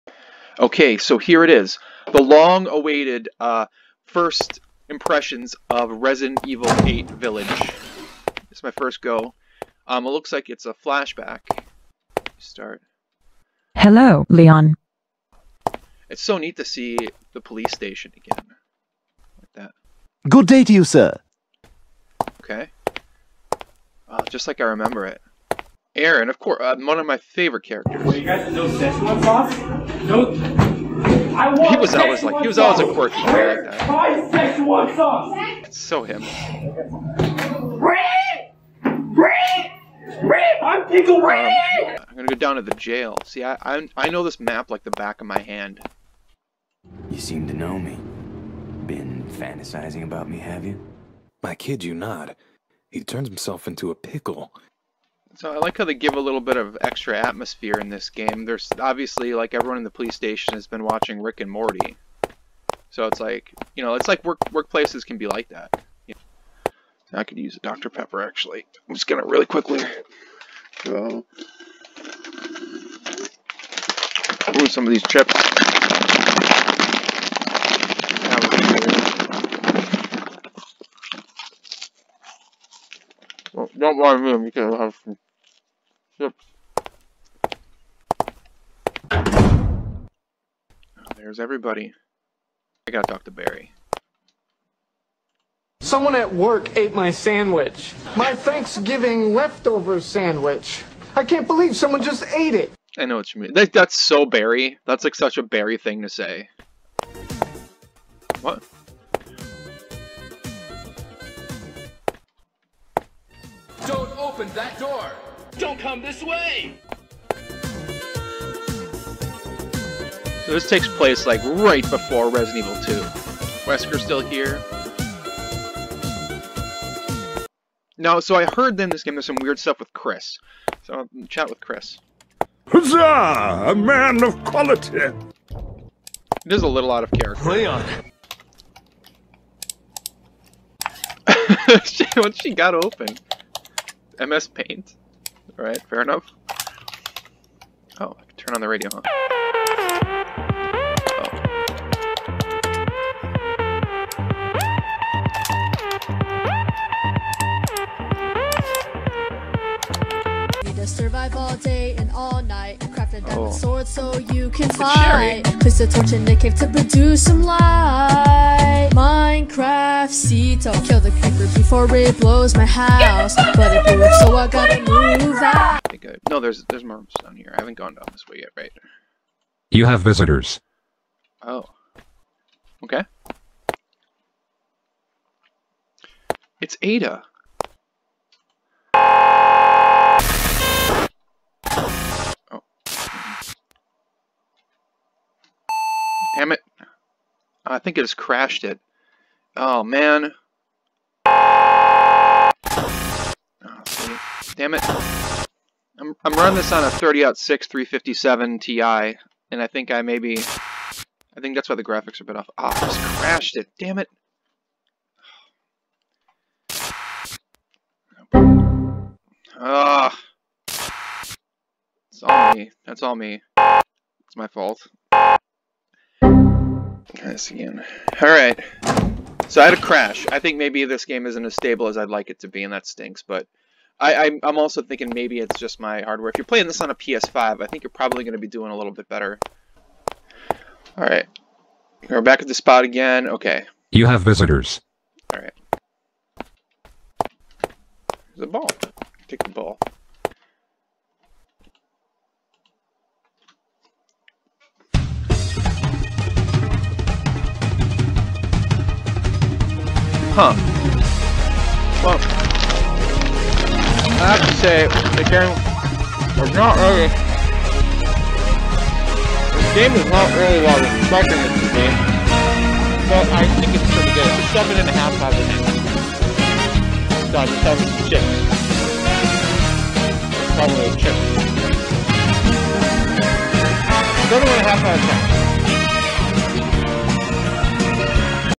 Resident Evil Village: Flawed, Not What We Expected... and fantastic.
My first impressions of Resident Evil: Village. It really is like nothing we could have expected, but is that for better or worse?